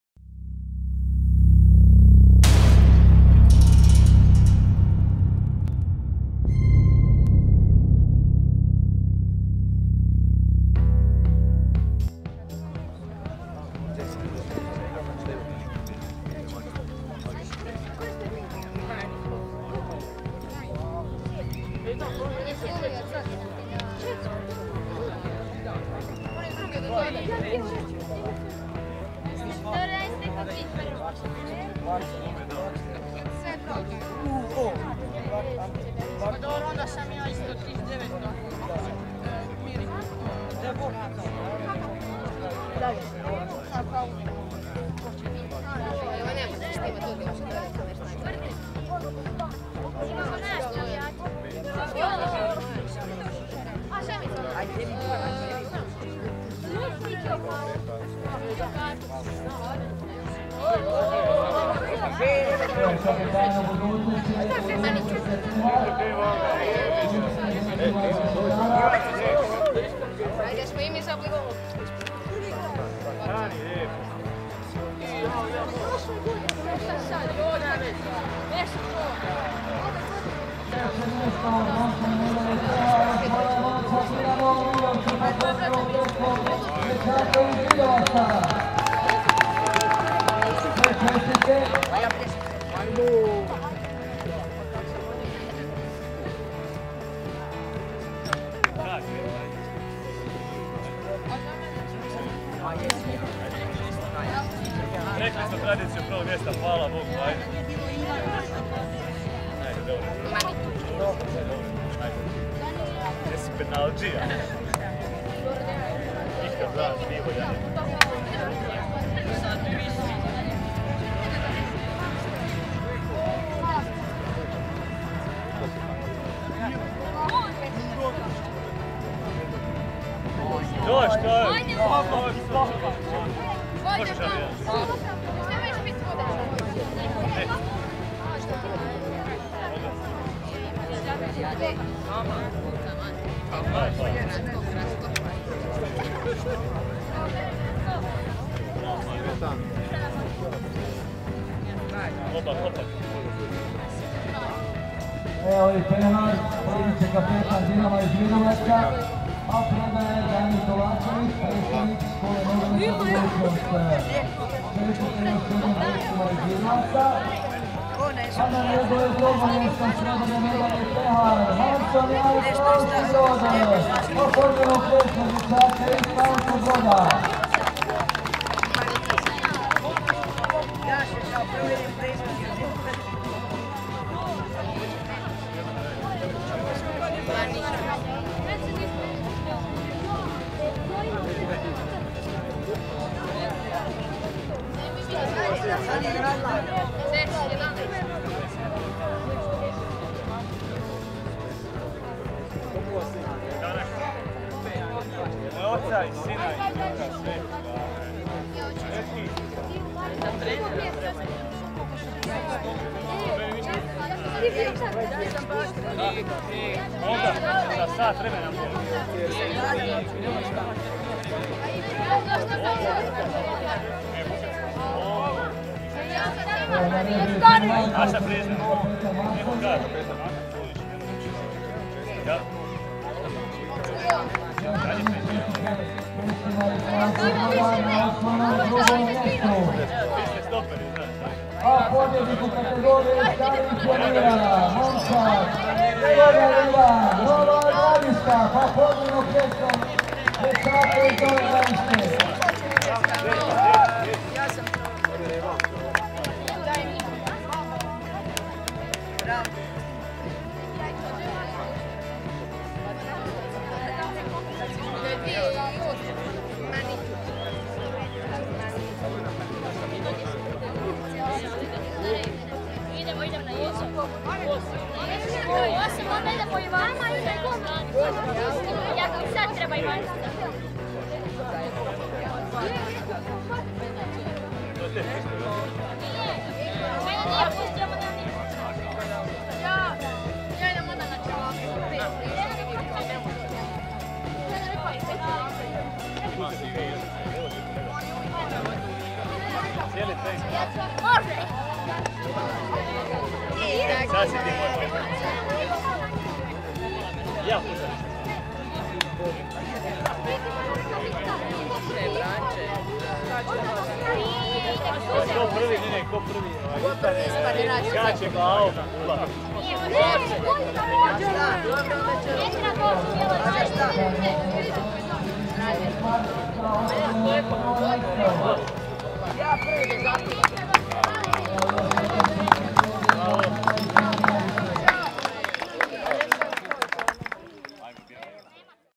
⭕️5. Dječji Nogometni Kup Budućnost 2020 # Stariji Pioniri # Dodjela medalja
5. Dječji nogometni kup Budućnost 2020. u kategoriji stariji pioniri održao se 22. kolovoza 2020. godine u organizaciji NK Budućnost Rešetari.

Dodjela medalja.

Iznad Svih!

Dinamo!